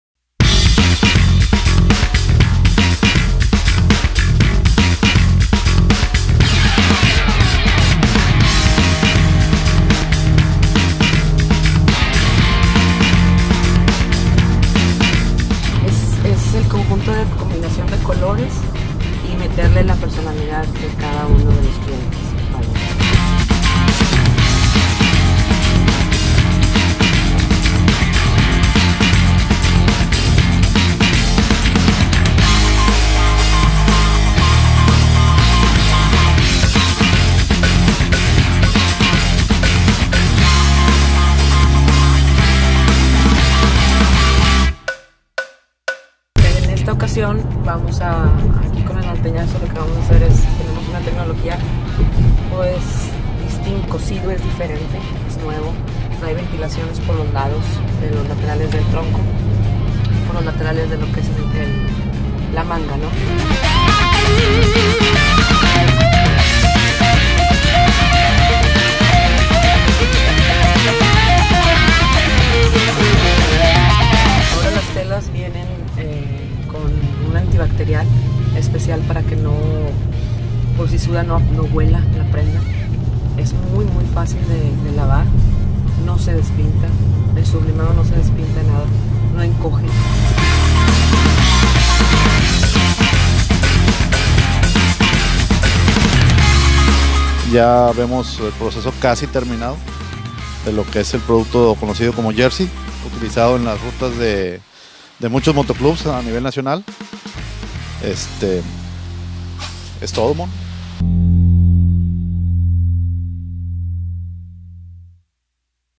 Visitando a L.A. Elemental, hacia el Norteñazo 2018
Uno de los patrocinadores oficiales del Norteñazo 2018, quien viste y le da imagen a este gran evento, nuestros patrocinadores también en Aventura Off Road TV, nos referimos a L.A: Elemental quienes nos invitan a conocer un poco de la mágia que hacen para ser de los proveedores más relevantes de jerseys y textiles deportivos del país.